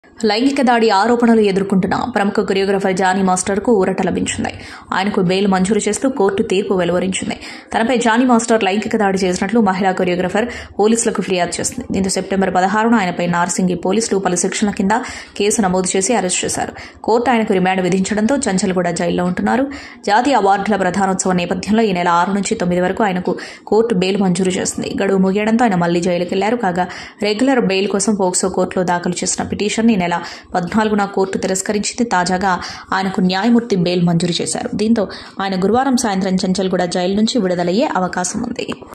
కొరియోగ్రాఫర్‌ జానీ మాస్టర్‌కు ఊరట.. బెయిల్‌ మంజూరుచేసిన కోర్టు
WATCH : కొరియోగ్రాఫర్‌ జానీ మాస్టర్‌కు ఊరట.. బెయిల్‌ మంజూరుచేసిన కోర్టు